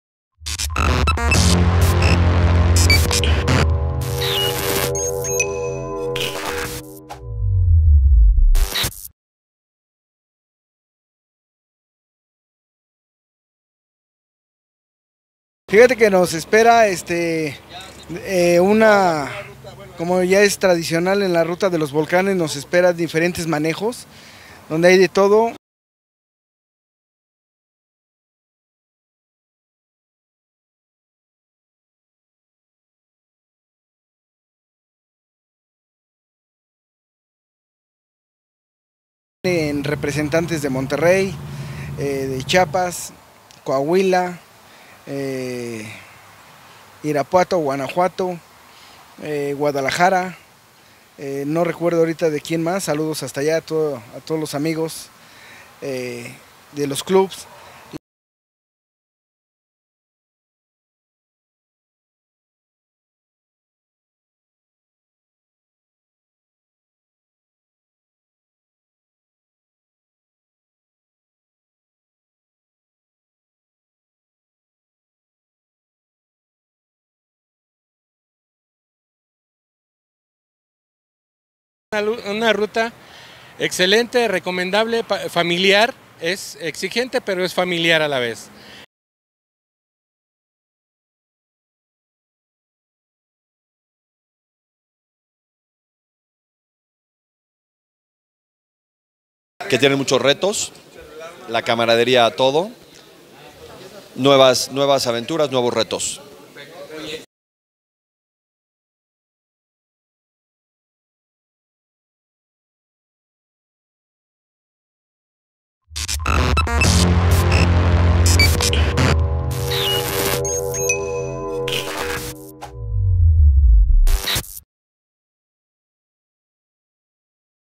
Reporte AORTV Ruta Volcanes, concentración de pilotos
Club TAE nos da la bienvenida a su X edición de la Ruta de los Volcanes, donde se iniciará en Metepec, Estado d e México para llegar a dormir a unas cabañas con vista ala Malinche y de ahí hasta Jalcomulco, llegando al pico de orizaba y el último día a las dunas de Chachalacas en Veracruz